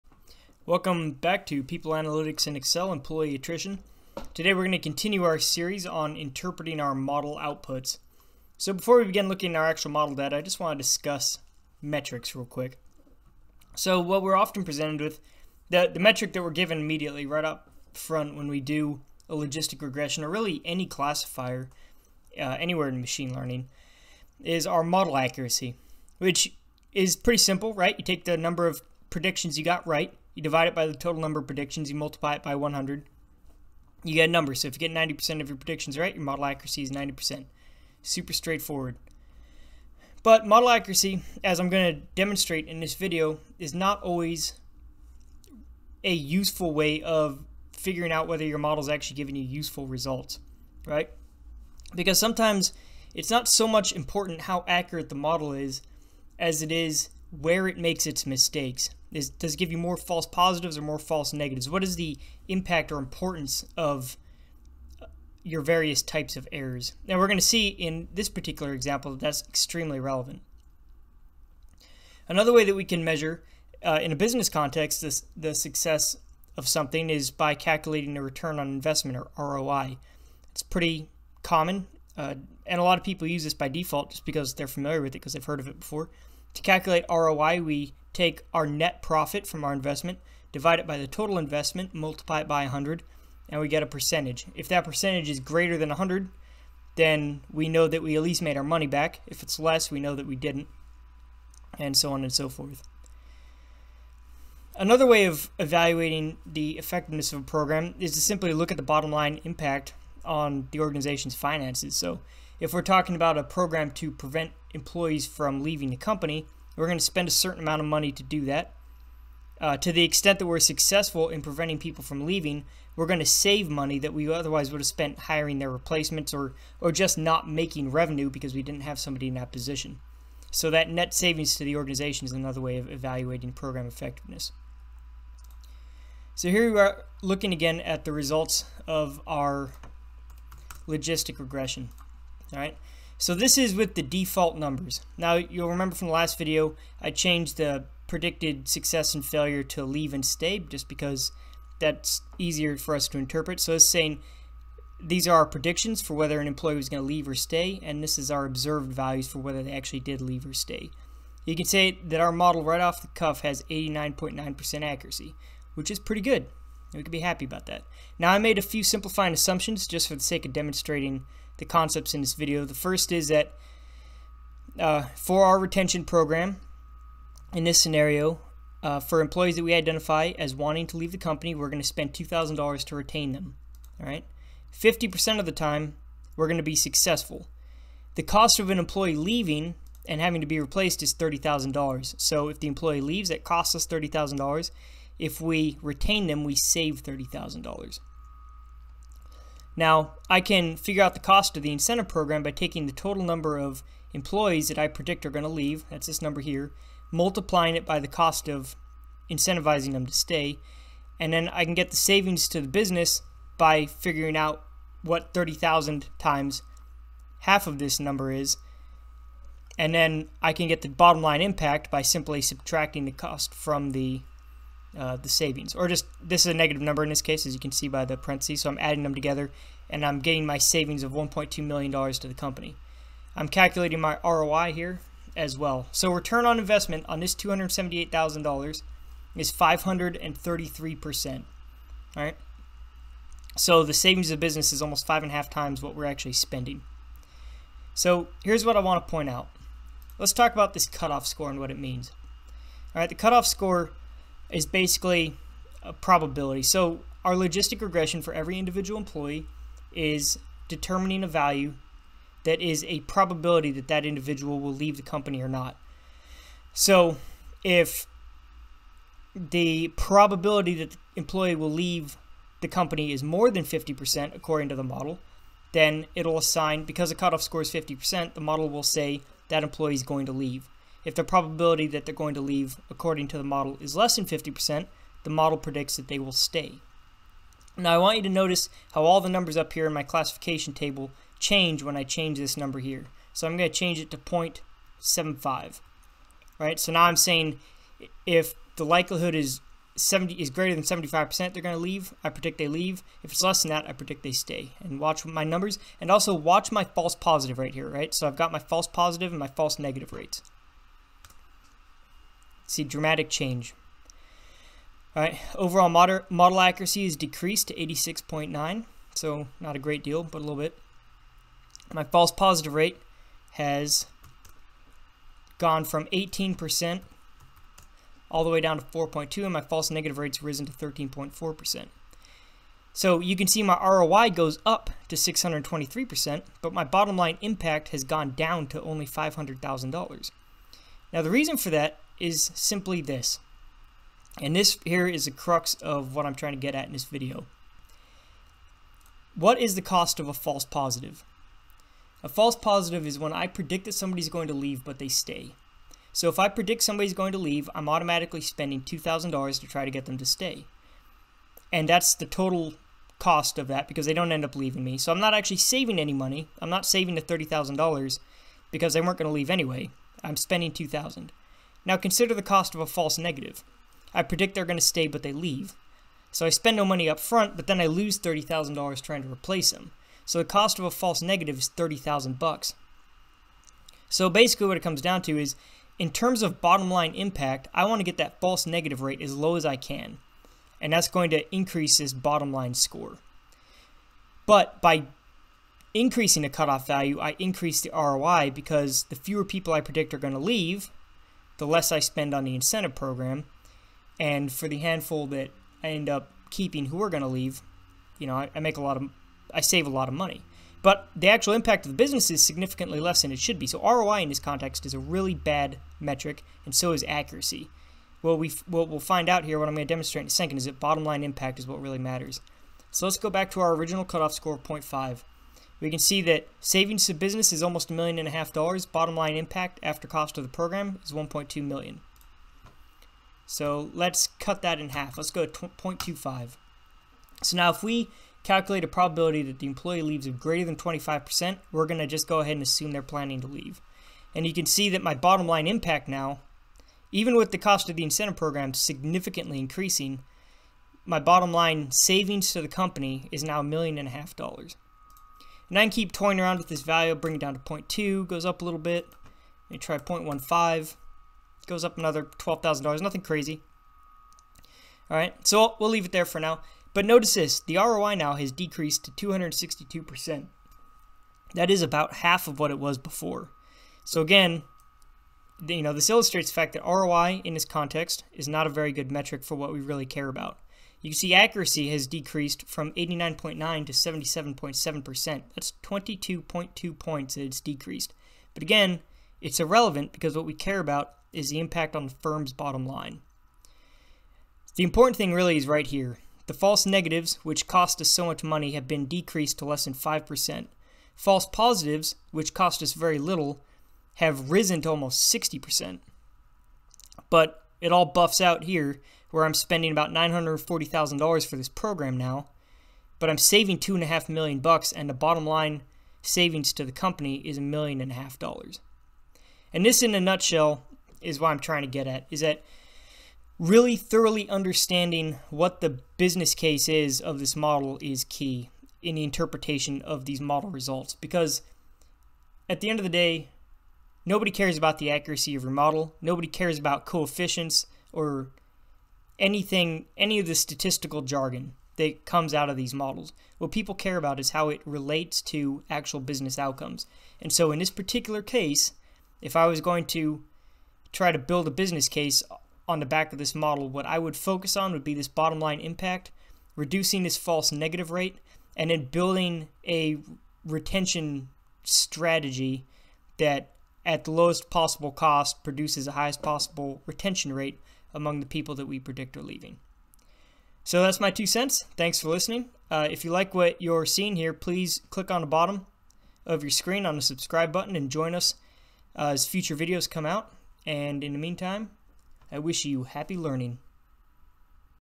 People Analytics in Excel: Employee Attrition (Interpretation Part 2: Choosing Meaningful Metrics)
In this video we continue to interpret the results of our logistic regression that predicts employee attrition from a fictional company. In our previous video we talked about how to intepret the confusion matrix that shows what predictions our model made and where it tended to get them wrong. In this video we use our confusion matrix to talk about understanding error rates is crucial to driving business results.

The crux of the matter is this: model accuracy is often irrelevant. A model that is 90% accurate might sound good, but if it is regularly reporting the wrong answer in a category that costs the company a lot of money, it is as good as useless. Depending on the business case a false positive can be a lot more expensive than a false negative, or vice versa. Understanding this and knowing how to tweak model parameters to create better business results is crucial. In this video we demonstrate this by changing the model's cutoff score and seeing how those decisions impact financial outcomes for our fictional business.

In the world of people analytics/hr analytics/business analytics, context is king. If we understand it, we can single-handedly deliver enormous competitive advantage to our employer.